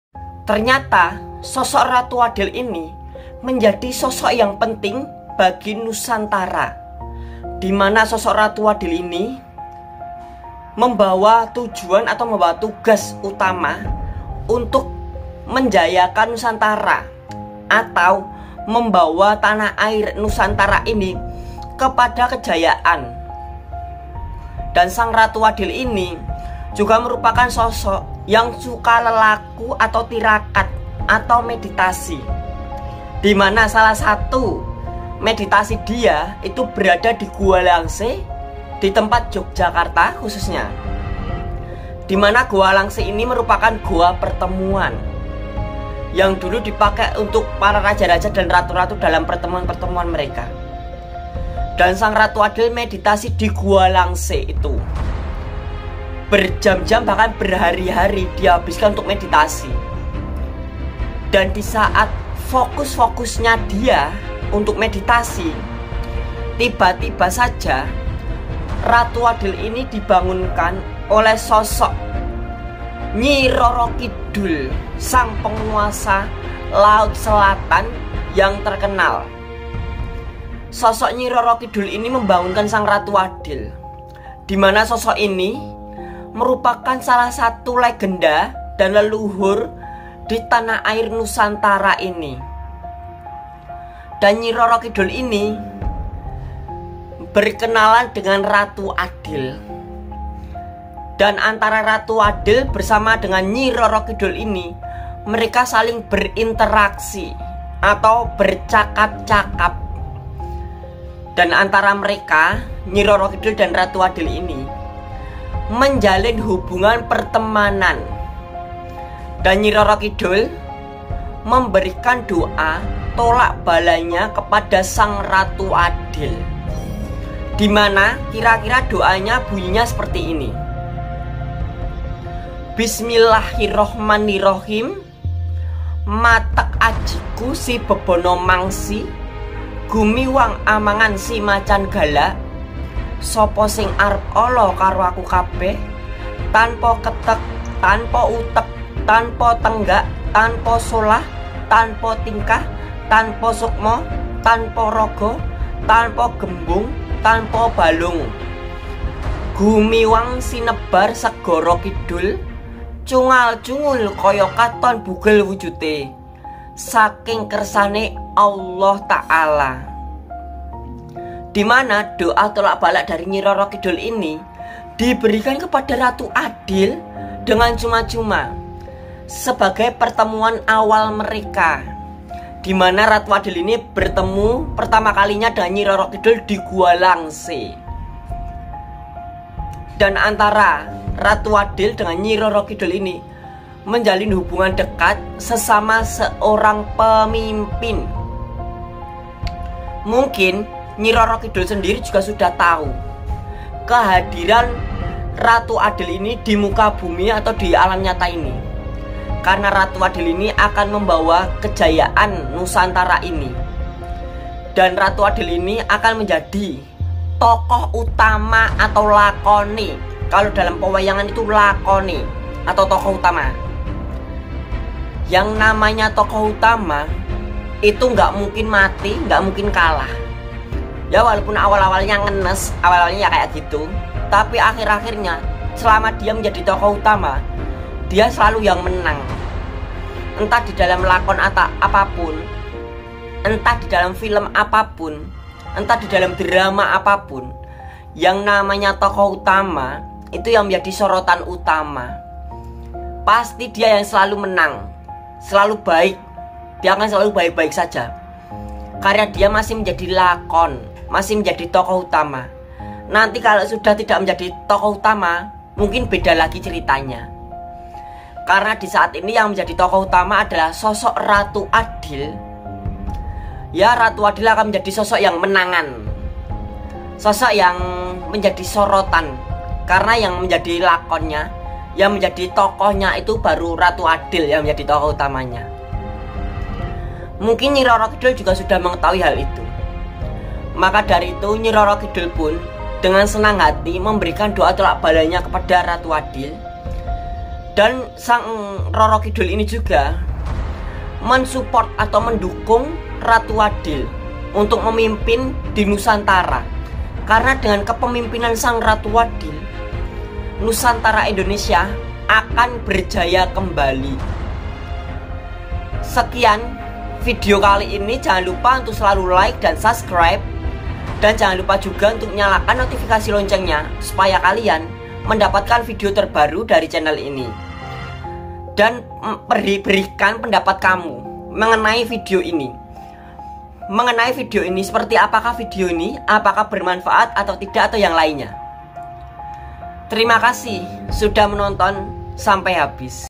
Ratu Adil VS Nyi Roro Kidul‼️@FaktaDuniaGaib
Vidio ini membahas mengenai pertemuan ratu adil dengan nyi roro kidul di gua langse.Dimana ini merupakan pertemuan pertama diantara mereka.
Dan Nyi roro kidul berikan hadiah kepada ratu adil mengingat pertemuan pertama mereka.
Dengan memberikan doa tolak bala segara kidul kepada ratu adil yang bunyinya seperti ini :

"Bismillah.
Matek ajiku sibebana mangsi,gumiwang amangan simacan galak.
Sopo sing arep ala karo aku kabeh
Tanpo ketek
Tanpo uteg
Tanpo tenggag
Tanpo solah
Tanpo tingkah
Tanpo sukma
Tanpo rogo
Tanpo gembung
Tanpo balung
Gumiwang sinebar segoro kidul.Cungal cungul koyo katon bugel wujudte.
Saking kersane Allah."

Dimana doa ini menjadi pemberian pertama dari nyi roro kidul bagi ratu adil.

Semoga saja vidio ini bermanfaat & menghibur kamu semua.
Dan nyalakan notifikasi loncengnya juga.

Berikan pendapatmu mengenai vidio ini dikolom komentar ya...